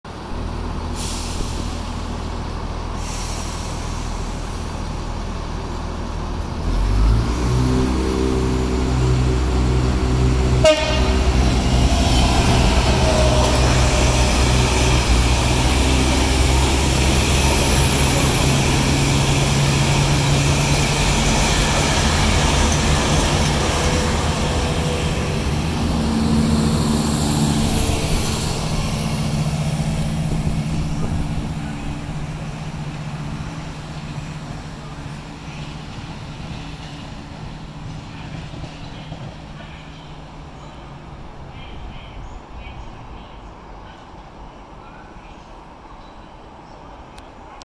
Freightliner Class 66 and Southern Class 171 at Ashford International
From Hoo Junction to Canterbury East, 66524 works 603D, while Working 2D23, 171805 Departs for Eastbourne